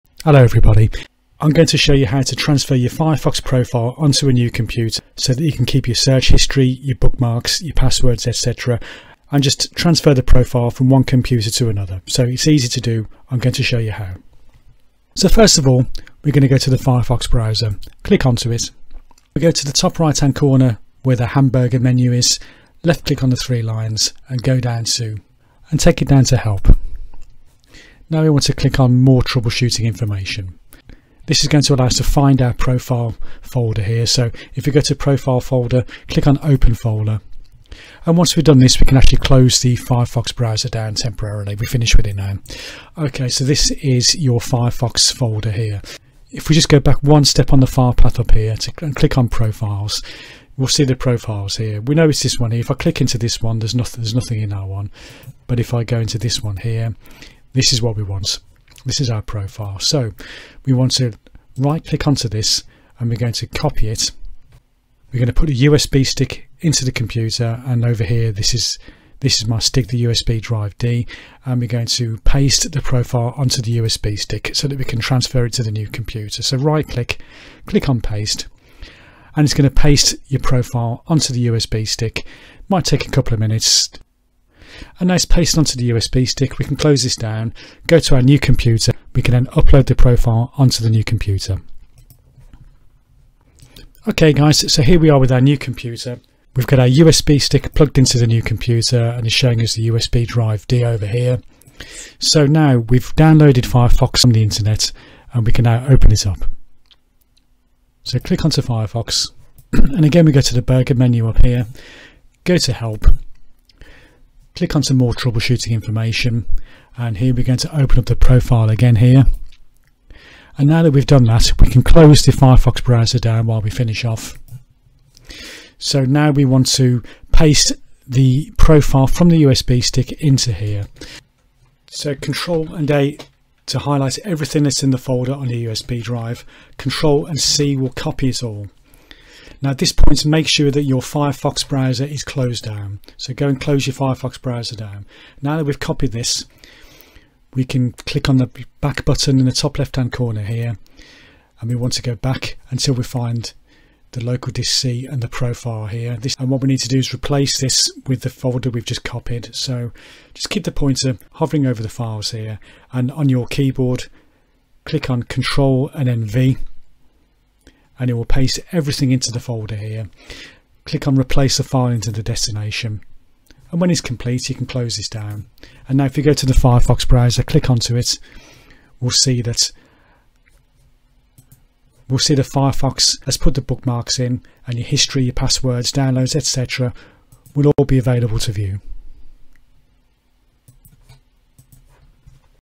How To Transfer Firefox Browser Profile To A NEW PC
If you've bought a new PC, you can transfer your Firefox Browser with all your settings to it.

This includes your -

PROFILE
BOOKMARKS
HISTORY
FAVOURITES
PASSWORDS

It's easy to do, and you will only need a USB stick to do it.

Follow this easy guide, and you'll be up and running within minutes.